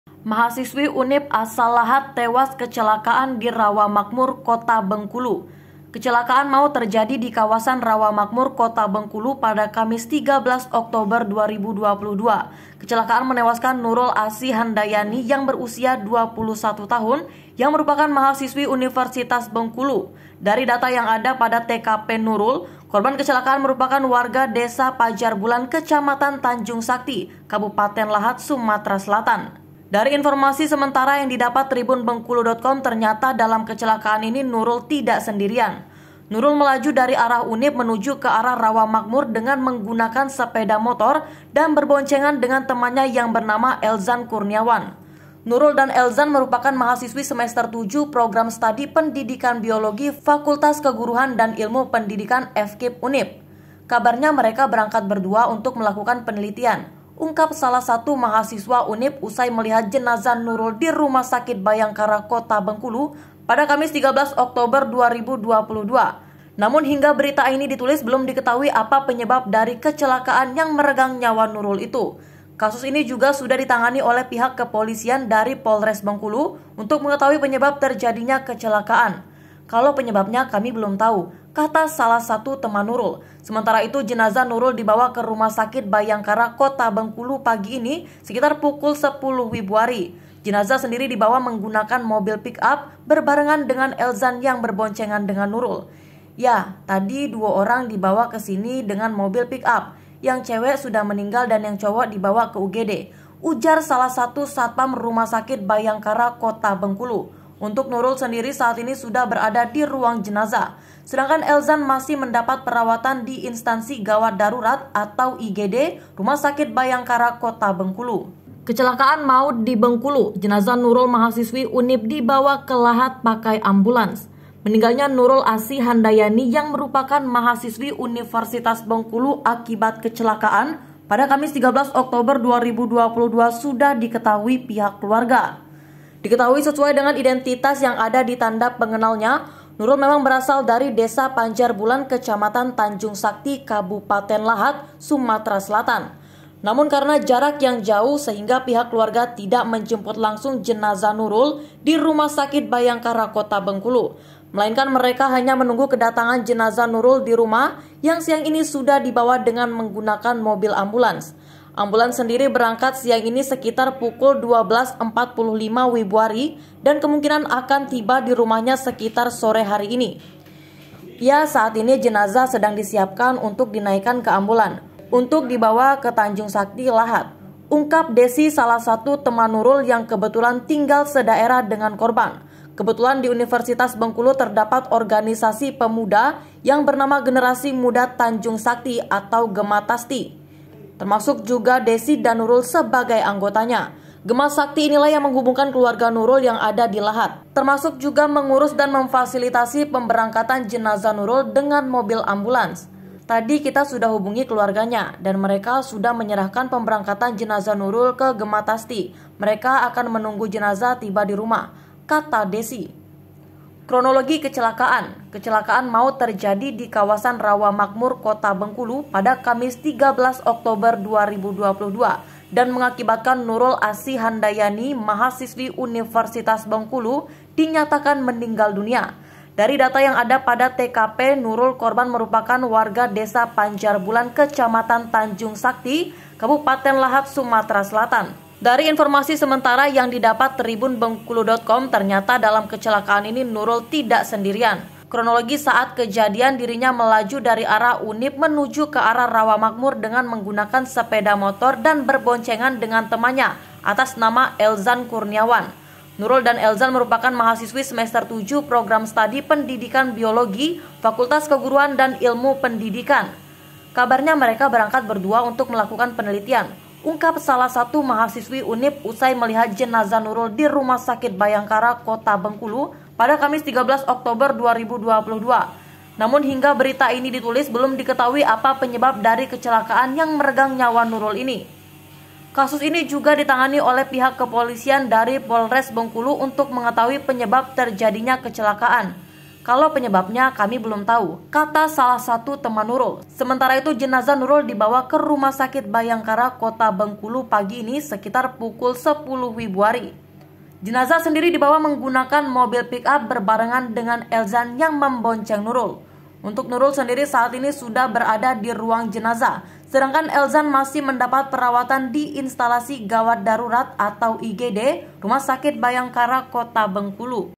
BREAKING NEWS: Mahasiswi UNIB asal Lahat Tewas Kecelakaan di Rawa Makmur Kota Bengkulu
TRIBUNBENGKULU.COM, BENGKULU - Kecelakaan maut terjadi di kawasan Rawa Makmur, Kota Bengkulu Kamis (13/10/2022).
Kecelakaan menewaskan Nurul Asih Handayani (21) yang merupakan mahasiswi Universitas Bengkulu (UNIB).
Dari data yang ada pada KTP Nurul, korban kecelakaan merupakan warga Desa Pajar Bulan Kecamatan Tanjung Sakti Kabupaten Lahat, Sumatera Selatan.